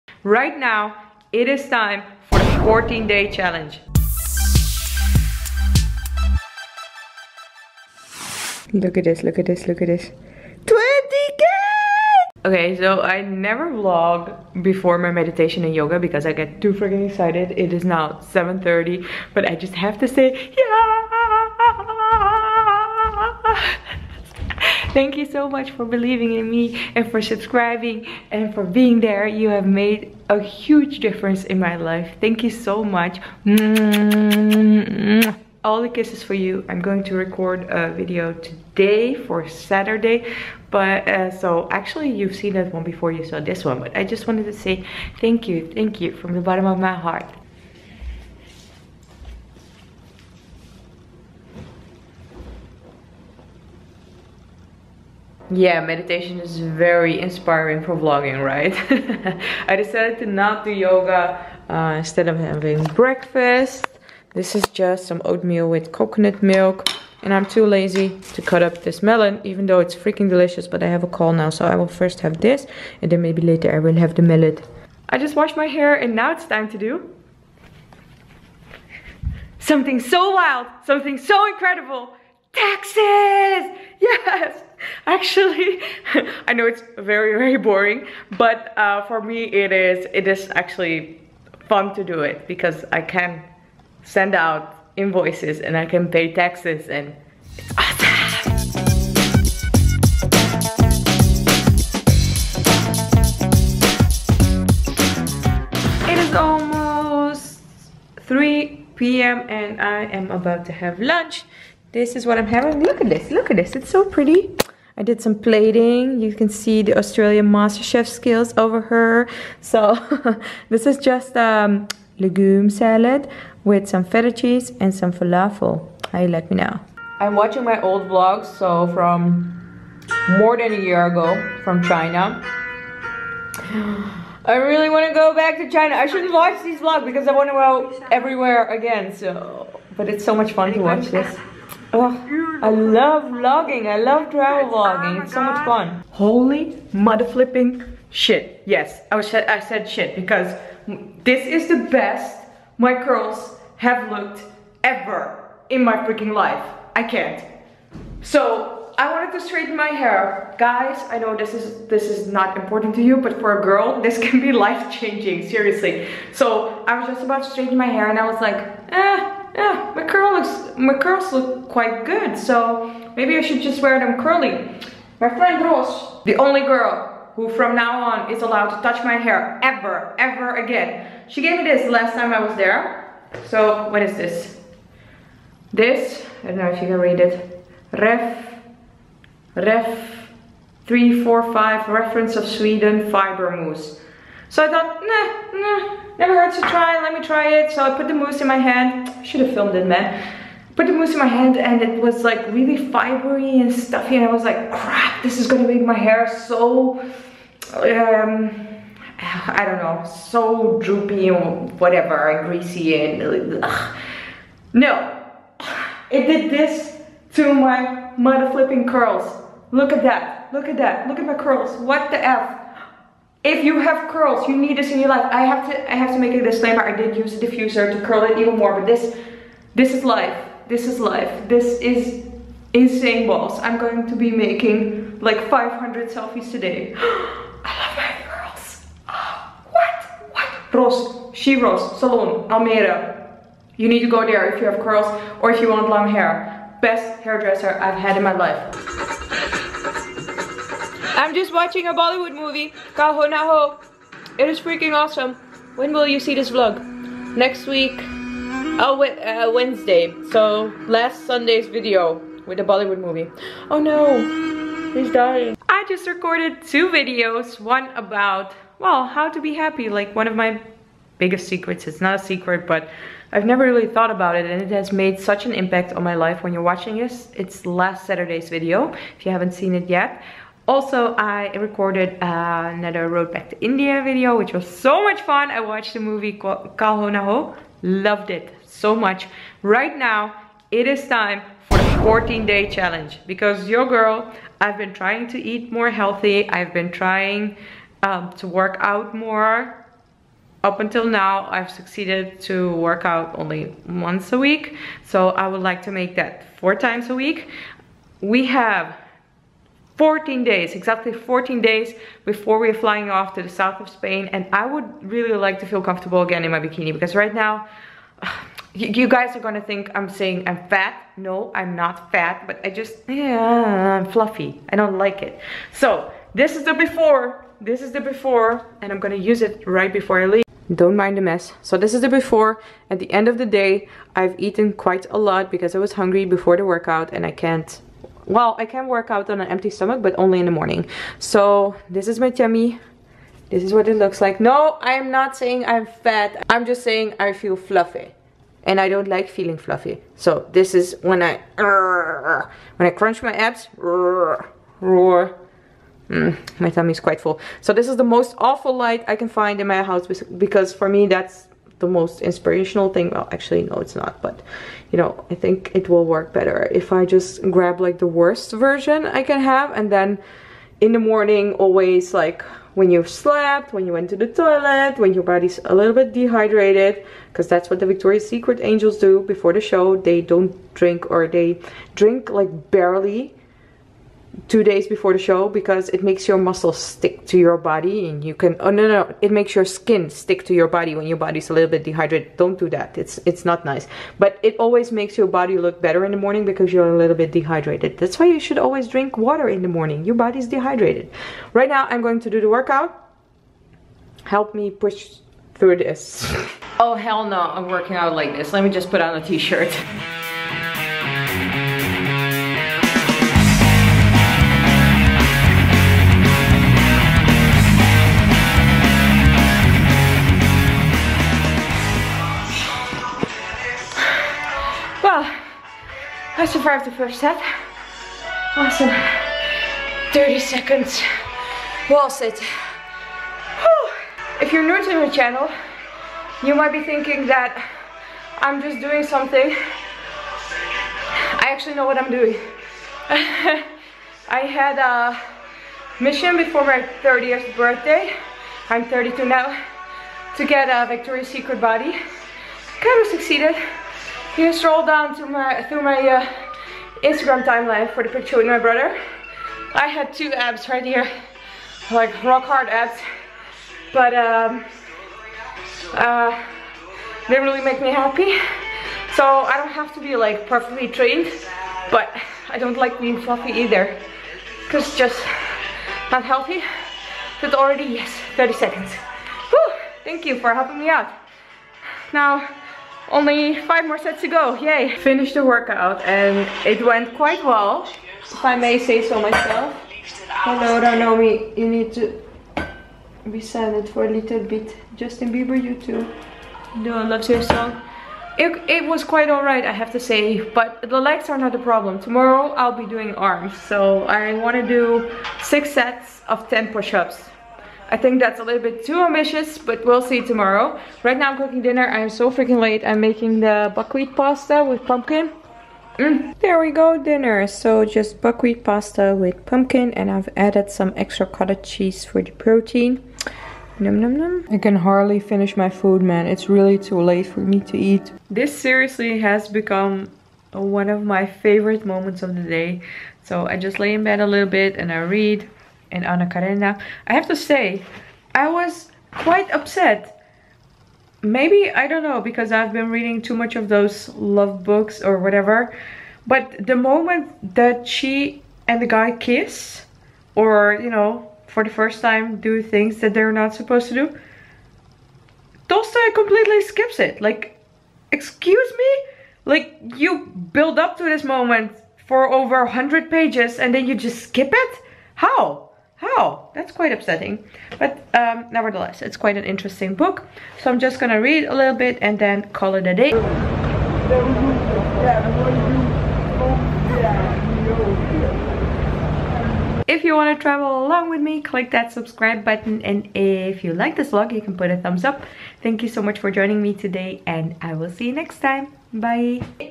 GET FIT AGAIN! - TRAVEL VLOG 397 AMSTERDAM | ENTERPRISEME TV
I'm about to get fit again! It's been a long time since I worked out but I really want to get fit again. We're leaving for Spain in exactly 14 days (7 when you're watching this) and bikini's are involved ;-). If you want to get fit again too let's do this together!